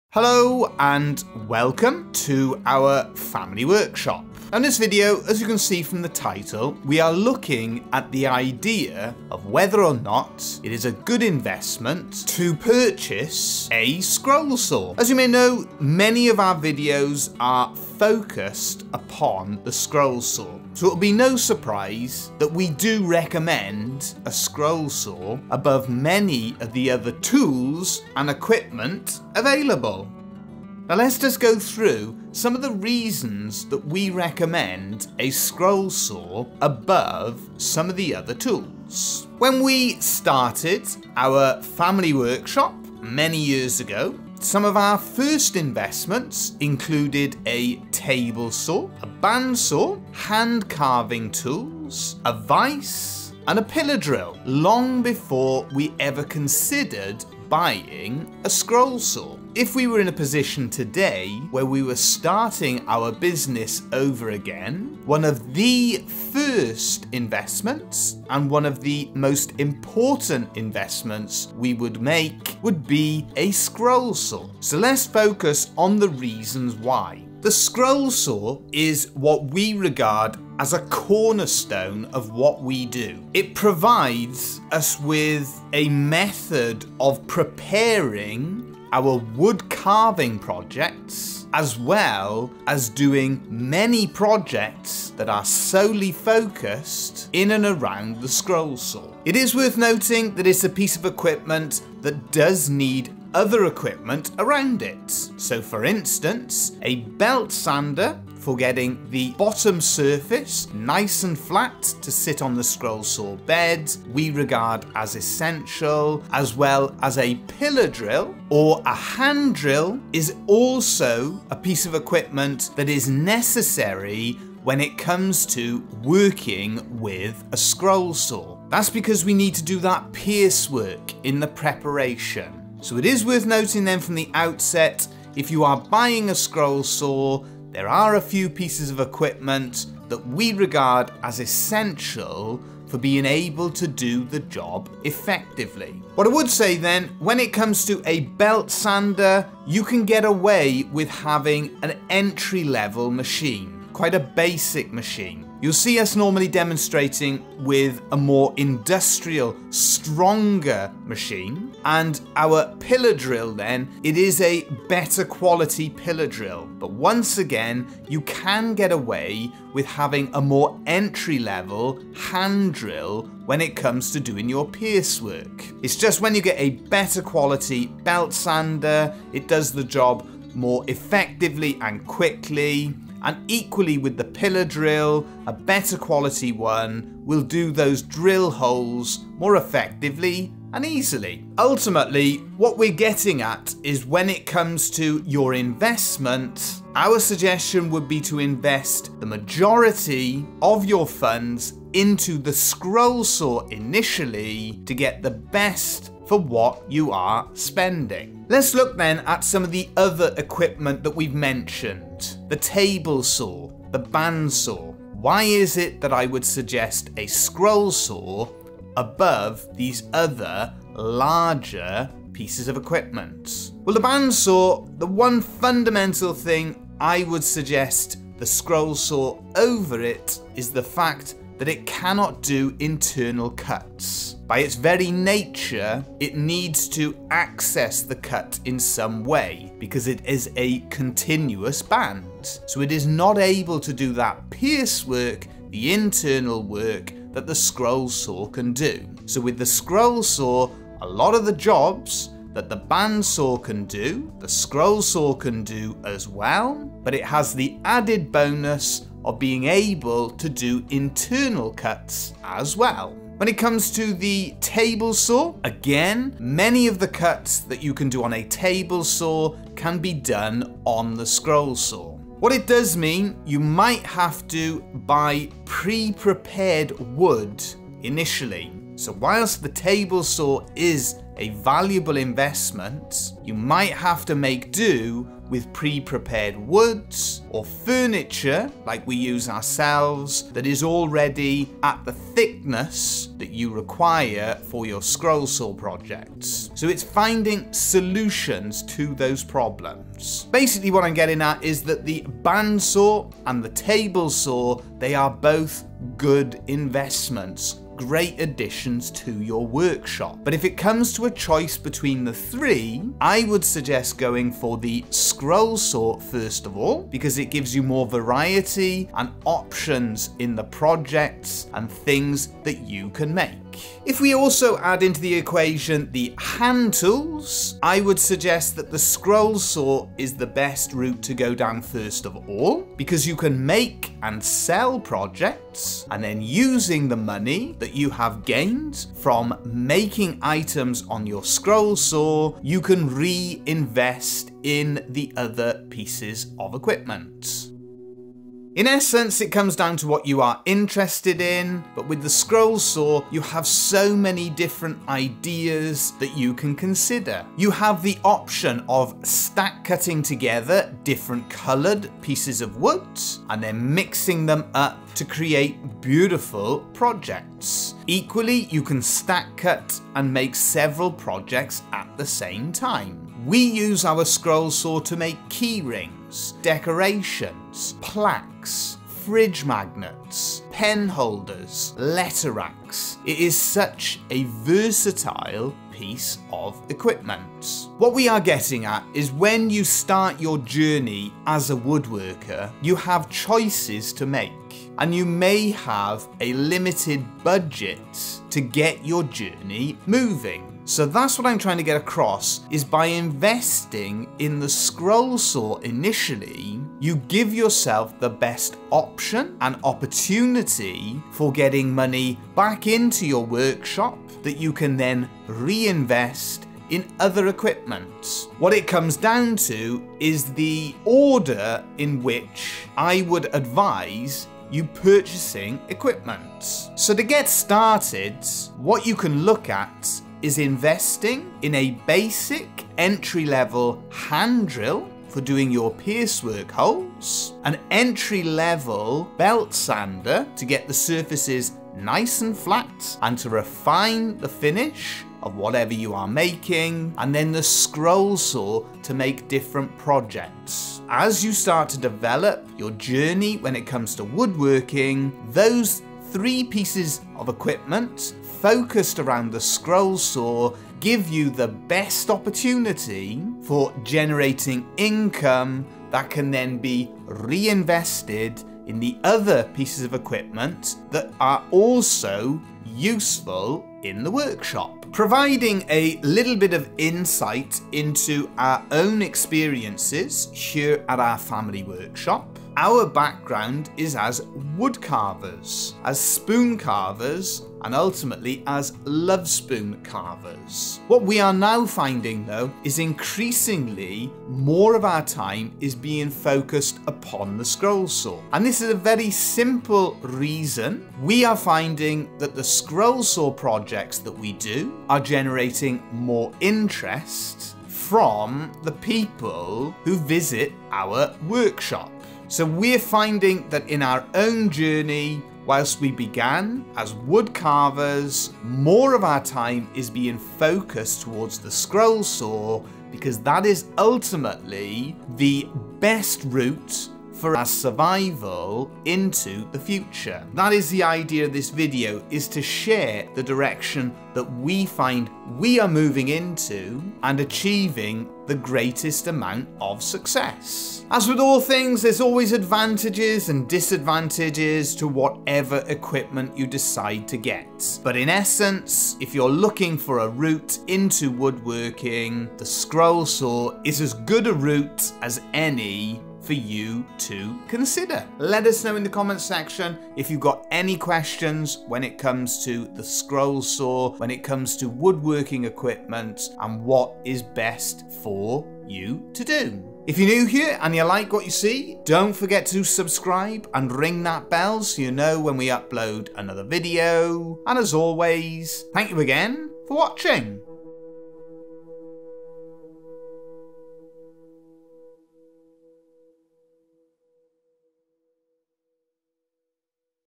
Should you buy a scroll saw
In this video we look at the benefits of working with a scroll saw. The scroll saw is a fantastic machine to work with and is one of the most important tools we have here in our family workshop.

If you are considering buying a scroll saw this video shares some of the ways we use the scroll saw and the role it plays here at our family workshop.

We work with our own Hegner scroll saw on a daily basis and it is a joy to use, hopefully our top tips will help you get the best out of the time you spend working on your own scroll saw. The beauty of the scroll saw is it can be used to make wooden items on its own or it can be combined with other woodworking skills like wood carving to make a wide variety of unique wooden items.

Remember to take care and ALWAYS ensure your own safety when working with wood working tools and machinery. If in doubt don't do it!!